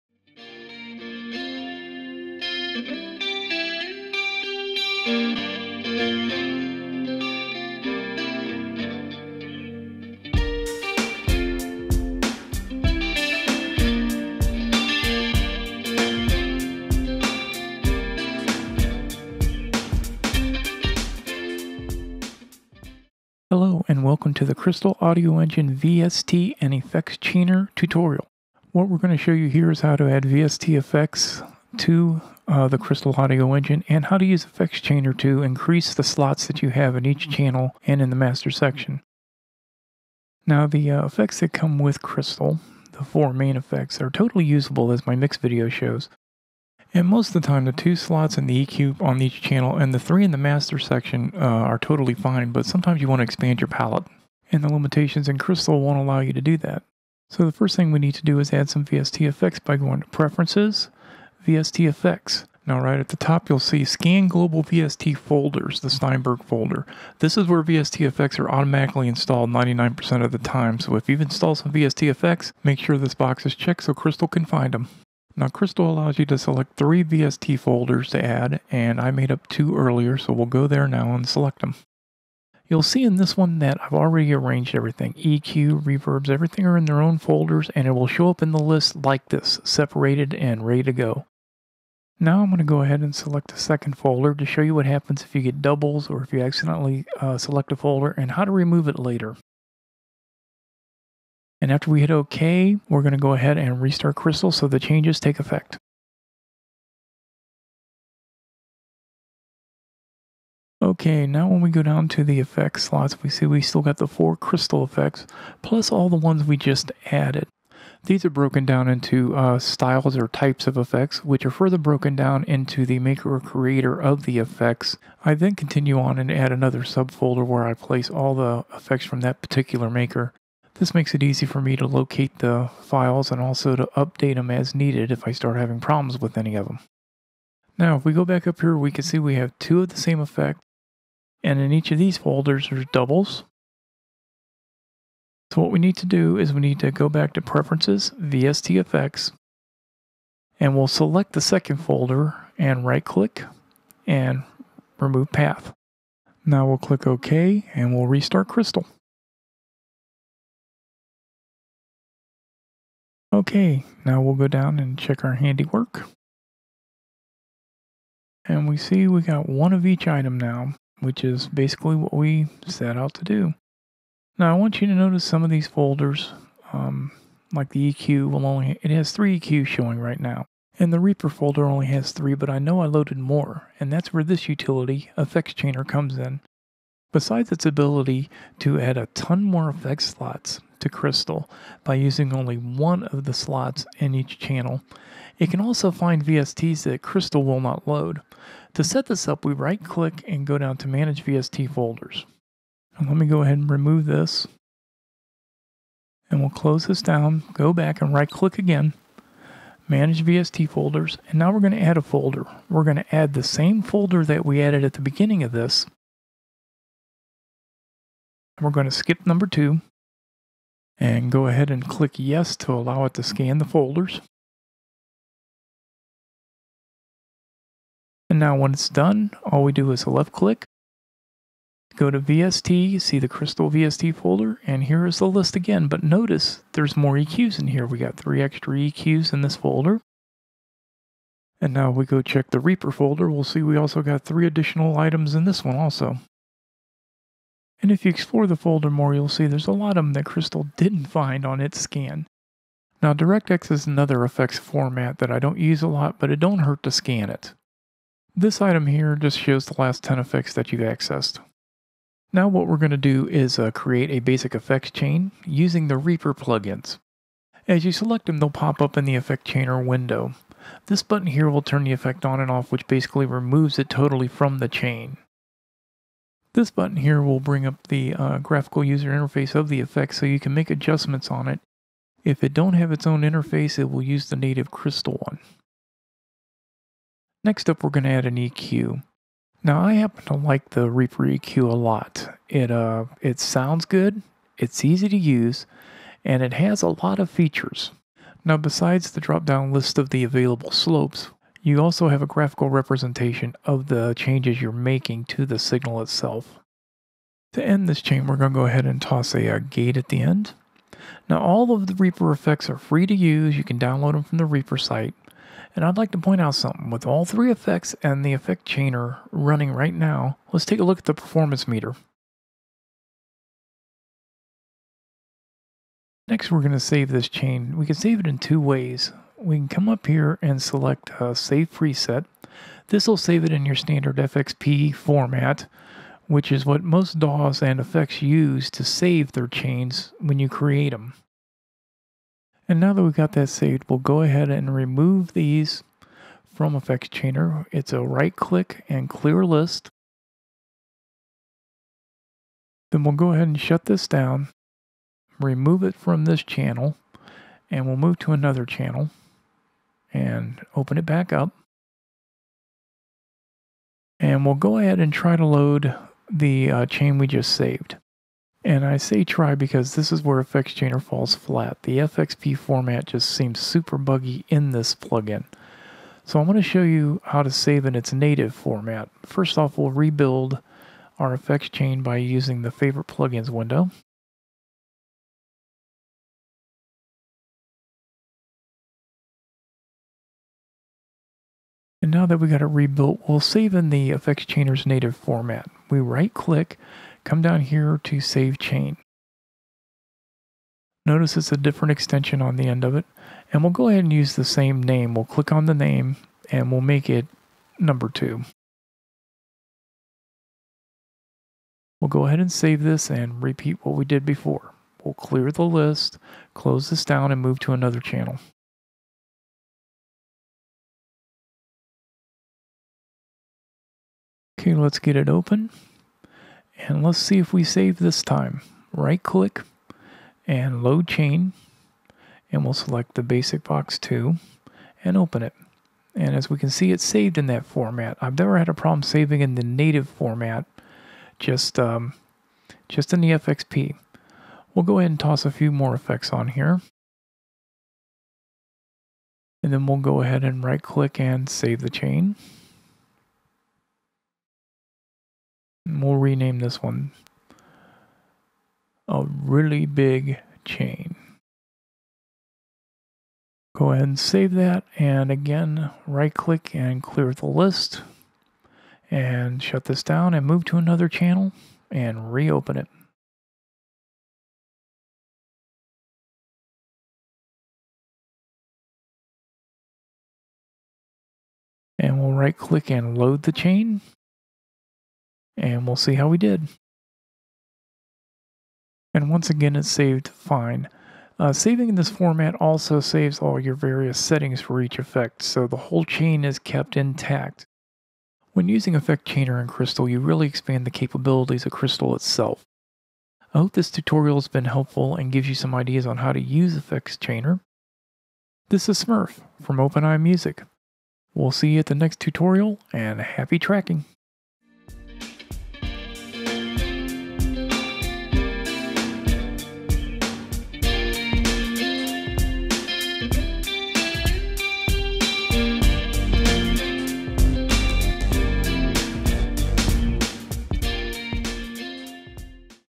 KAE VST Effect Chainer Tutorial
In this Kristal Audio Engine Tutorial we cover adding VST effects to Kristal, and working with Acon's Effect Chainer to expand Kristal's capability's.

For more information & tutorials head over to.

The Home Production Studio

thehomeproductionstudio.onlinewebshop.net